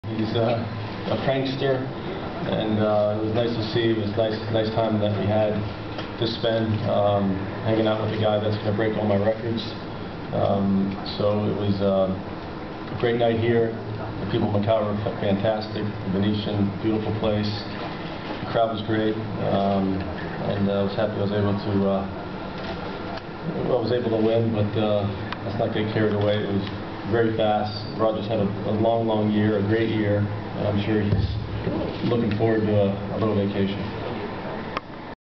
Pete Sampras post match presscon Venetian Macao
A brief clip of the post match press conference.  The players were asked to describe what the week has been like.  Sampras describe Federer as a prankster and that it was nice to hang out with the guy who's going to break his record.